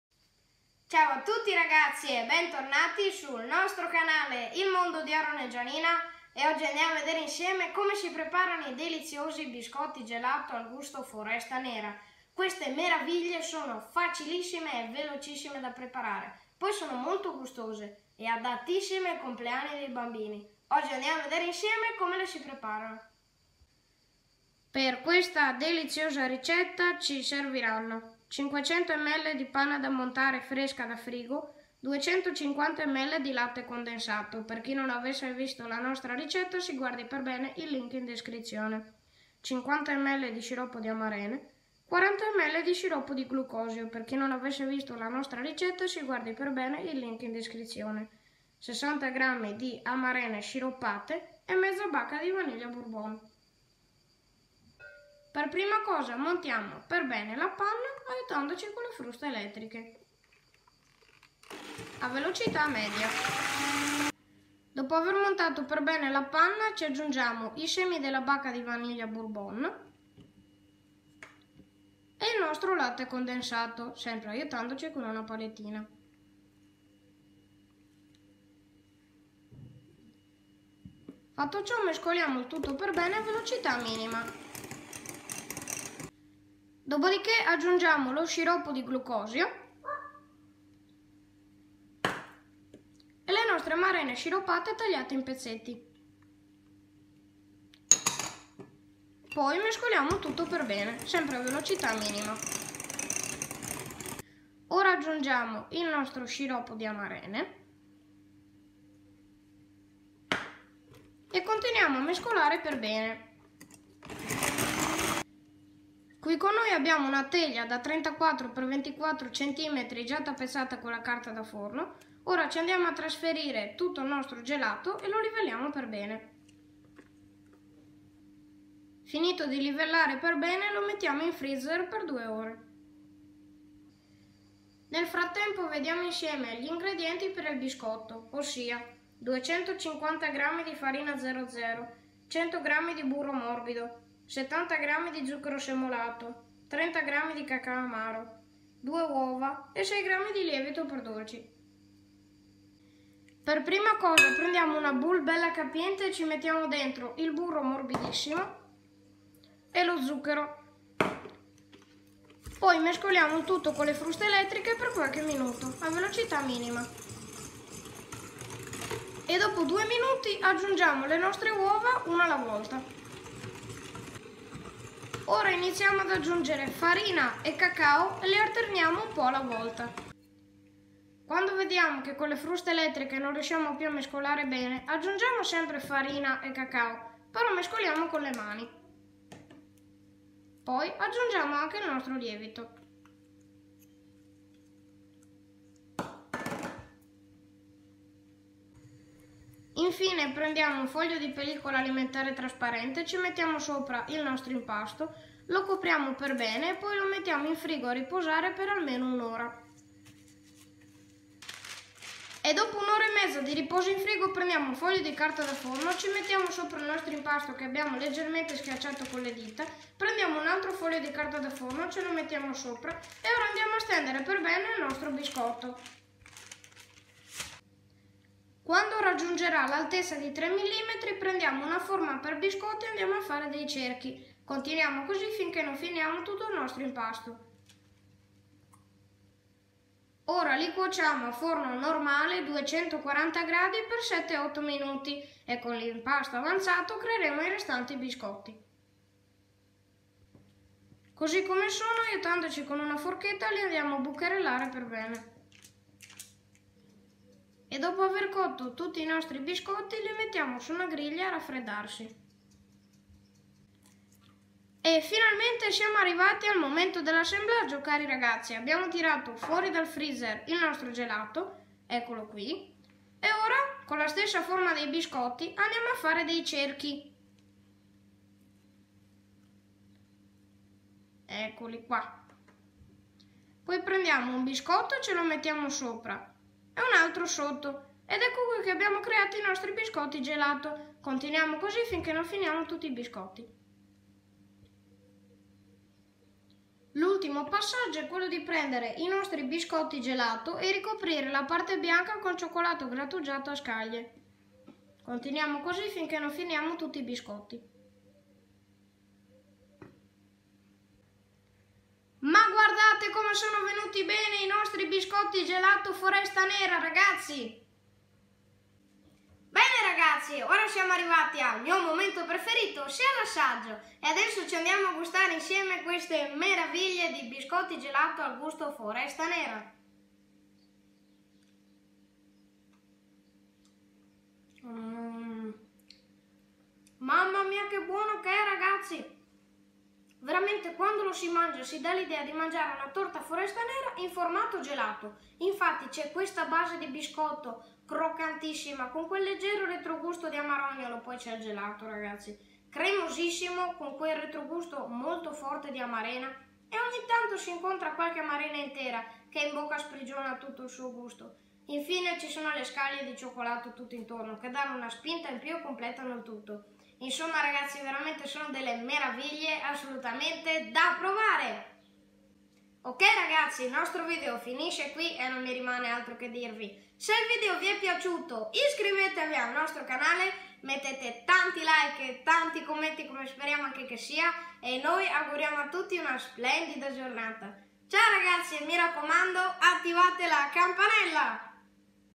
GELATO BISCOTTO FORESTA NERA facile e veloce senza gelatiera !
BUONGIORNO A TUTTI, CI SCUSIAMO MOLTO PER L'ERRORE DAL MINUTO 4:00 AL 4:16, POICHÈ I 2 CLIP SONO INVERTITI, PER UN ERRORE NEL CARICAMENTO DEL FILE. SIAMO MOLTO SPIACENTI.
Ciao a tutti, visto che il caldo non ci dà tregua abbiamo deciso di rinfrescarvi con un favoloso gelato: IL GELATO BISCOTTO AL GUSTO FORESTA NERA. Questa ricetta è velocissima, semplicissima da preparare, adatta ai compleanni dei bambini o come snack pomeridiano. Il suo gusto deciso ci ricorda la nostra torta foresta nera, però in formato gelato.

Per questa ricetta ci serviranno:
PER IL GELATO:
500ml di panna fresca fresca da frigo
250ml di latte condensato
50ml di sciroppo di amarene
40ml di sciroppo di glucosio
60g di amarene sciroppate
mezza bacca di vaniglia bourbon

PER IL BISCOTTO:
250g di farina "00"
100g di burro morbidissimo
70g di zucchero semolato
30g di cacao amaro
2 uova
6g di lievito per dolci